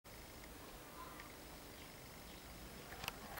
cat hiding in the bush
cat hiding. Oh! I can't catch this cat!